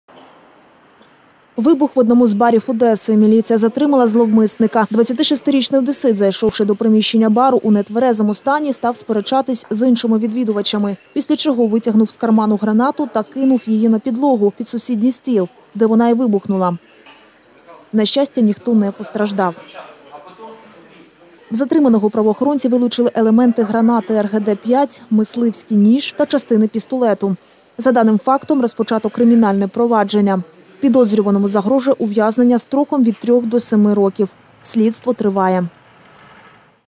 Вибух у барі на Щорса. Подробиці інциденту
Вибух в одному з барів Одеси. Міліція затримала зловмисника. 26-річний одесит зайшовши до приміщення бару у нетврезому стані став сперечатись з іншими відвідувачами. Після чого  витягнув з карману гранату та кинув її на підлогу, під сусіднй стіл, де вона і вибухнула. На щастя ніхто не постраждав.

В затриманого, правоохоронці вилучили елементи гранати «РГД-5», мисливський ніж та частини пістолет «Вальтер». За даним фактом розпочато кримінальне провадження. Підозрюваному загрожує ув'язнення строком  від трьох до семи років. Слідство триває.